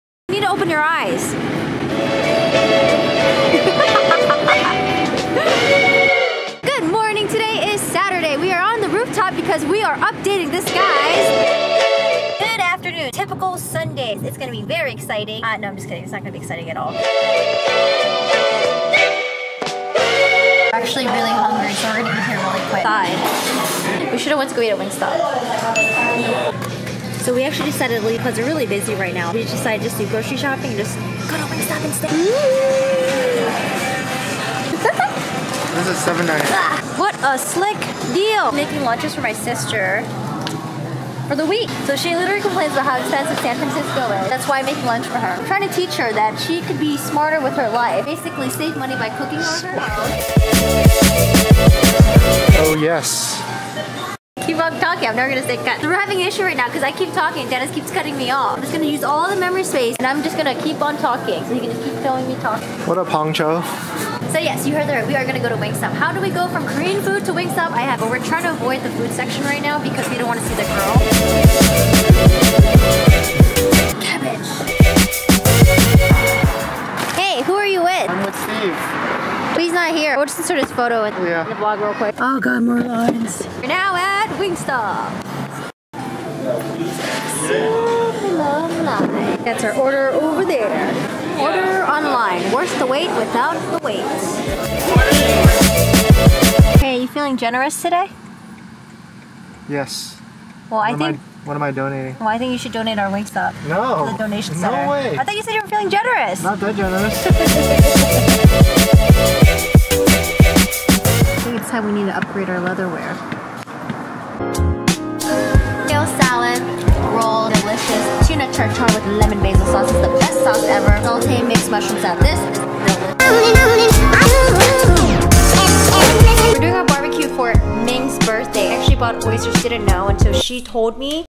KOREAN FOOD or WINGSTOP? || Grocery Shop RANTS!
ranting at korean grocery store and wingstop lies
typical corgisaan sunday funday with alisa grocery store rants

IF YOU LIKE GAMING

►►snap me: corgisaan